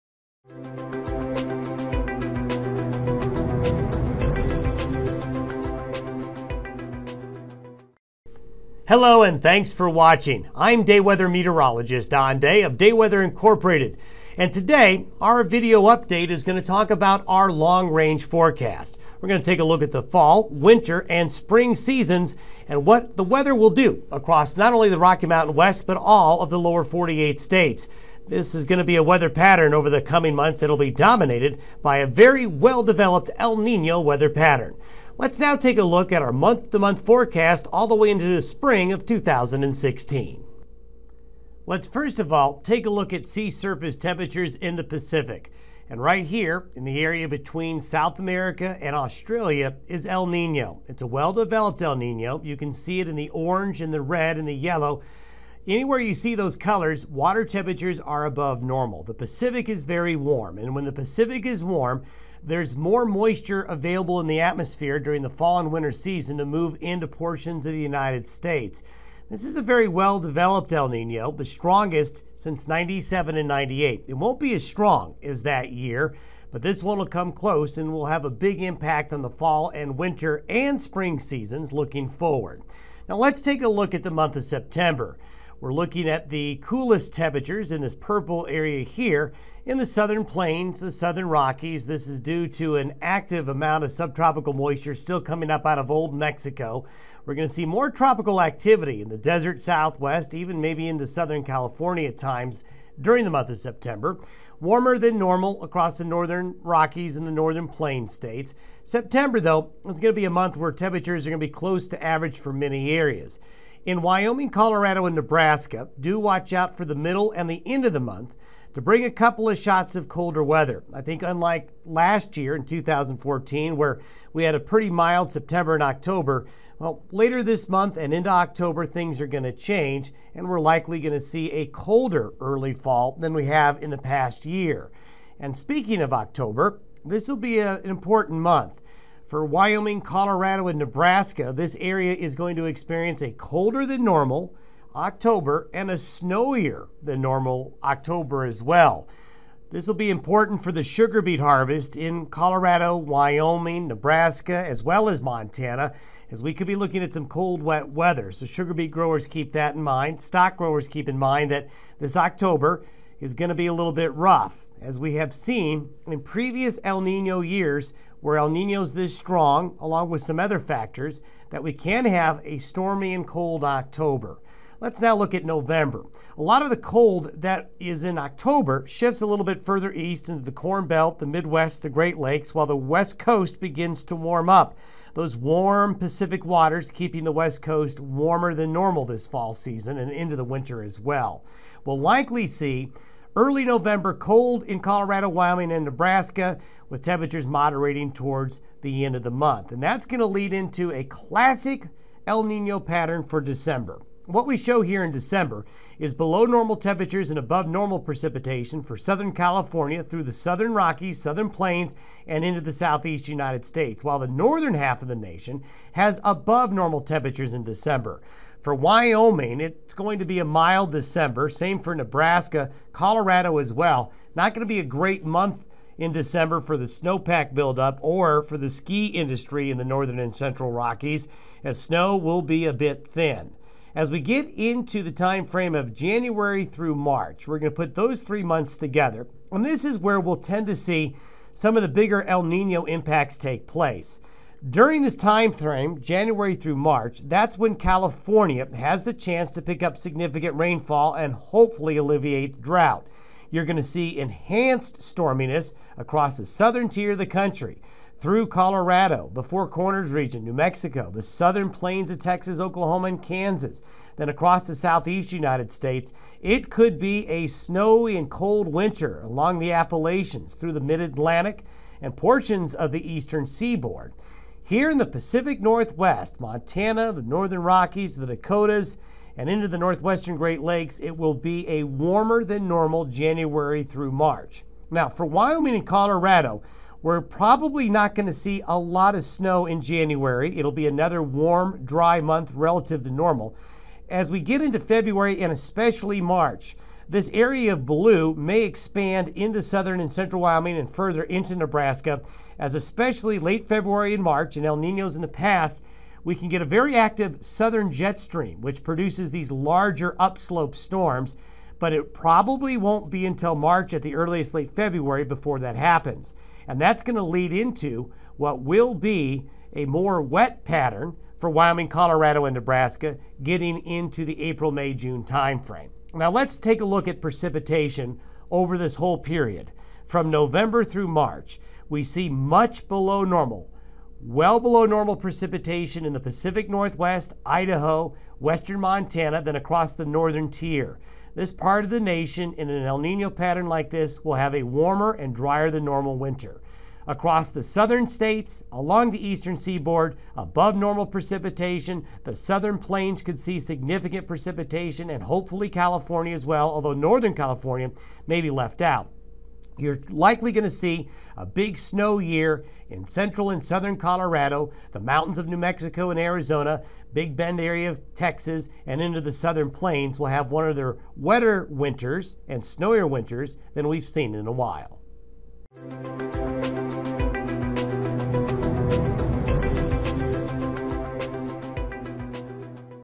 Long Range Outlook 2015-16
DayWeather meteorologist Don Day takes a look at the 2015/16 fall and winter outlook